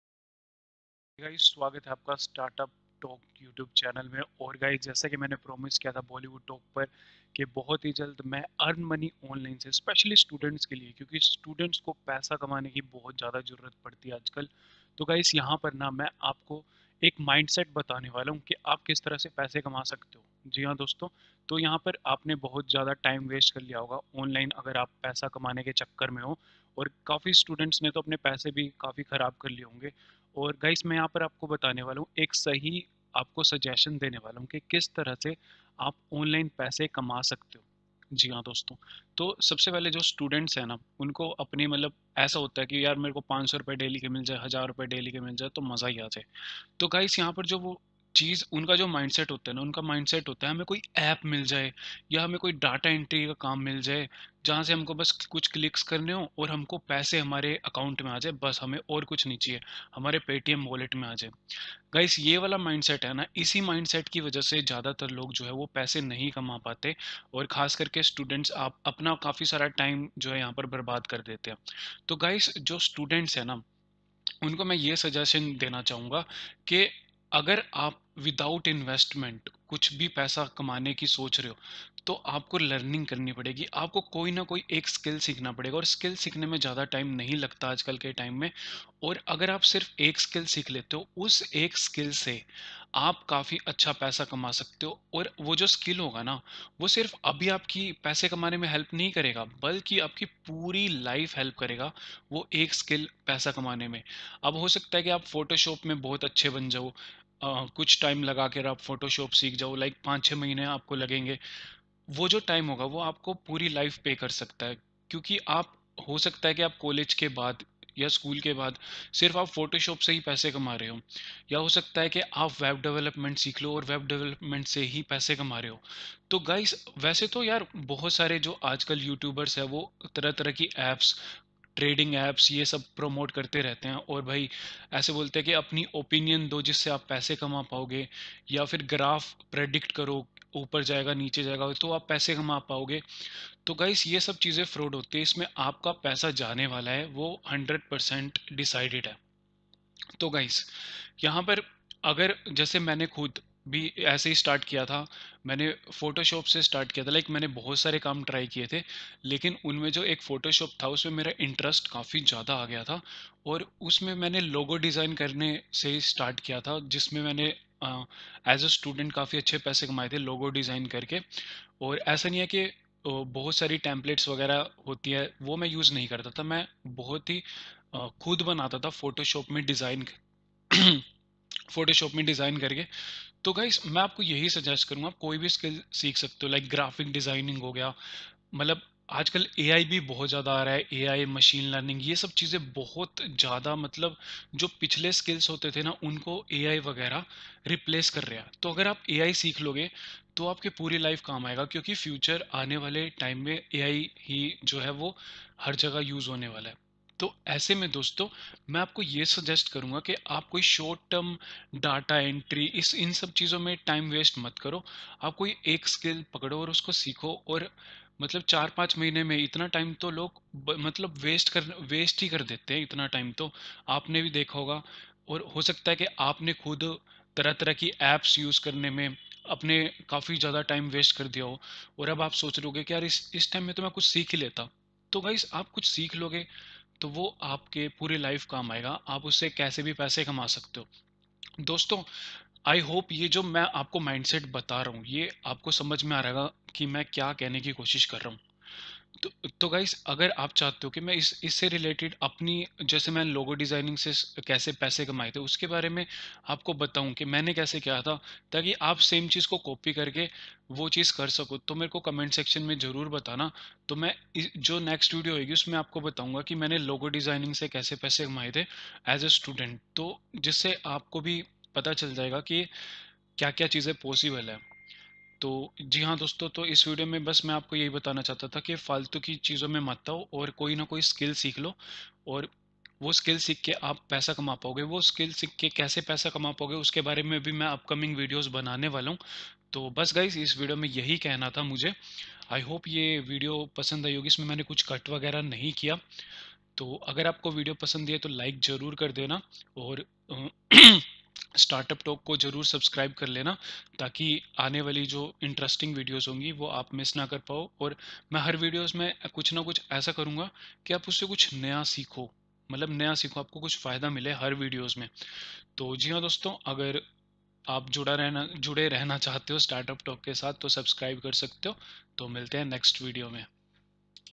(Mindset) Earn Money Online for Students in India without Investment
Hey friends! 🤑 Want to learn how to earn money online as a student in India without spending a single rupee? Well, you've come to the right place! 😎

In this video, we'll talk about:

How to find data entry opportunities for students.
Learning new skills without investing any money upfront.
Exploring fields like web development and design for free.
Beware of those tricky trading apps that can sometimes be shady!
My personal journey from starting with logo designing, and I'll share tips on how you can do it too! 🎨